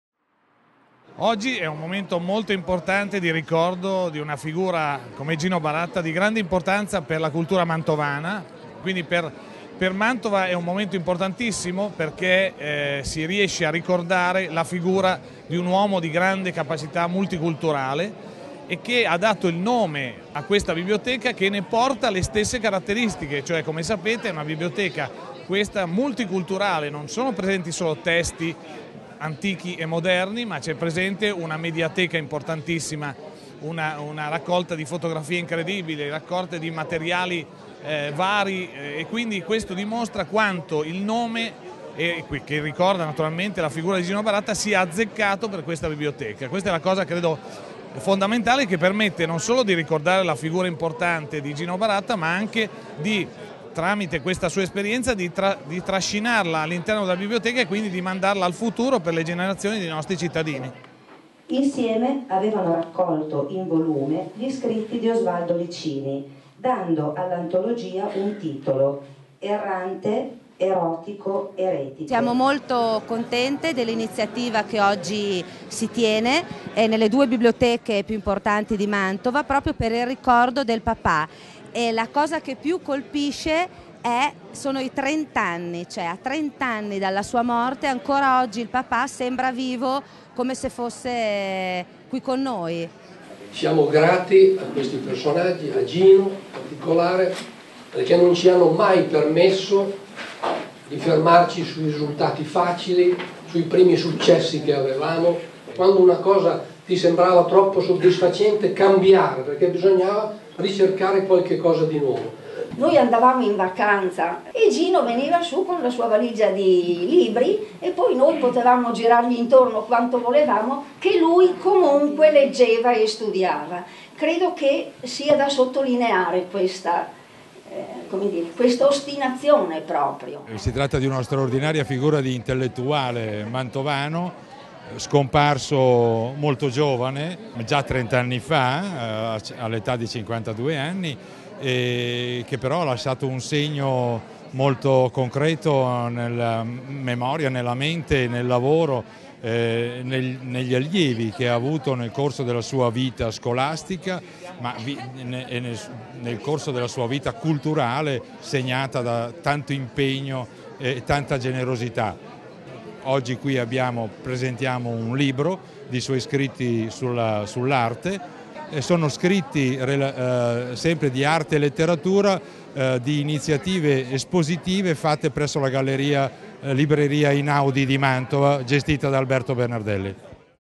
L'impegno culturale di Gino Baratta (Mantova, 24/01/2014)
A 30 anni dalla sua scomparsa il mondo bibliotecario ha ricordato la figura dello studioso e critico d'arte mantovano. Video realizzato dalla WebTV "Mantova.TV", solo su Internet al sito